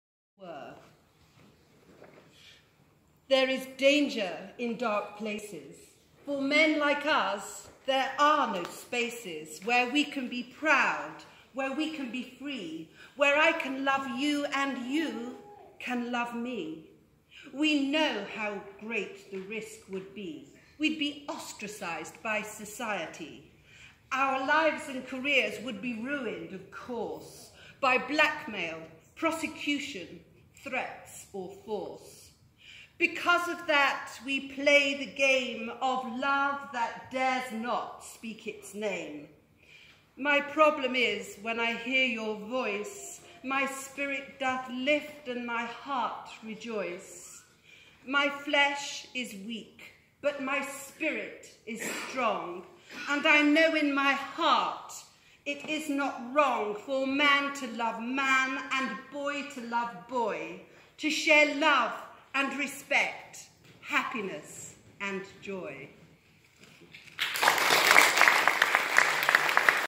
Camden Town Hall performance
Celebrating the partial decriminalisation of the Sexual Offences Act 27th July 2017 @ Mayor of Camden's chambers.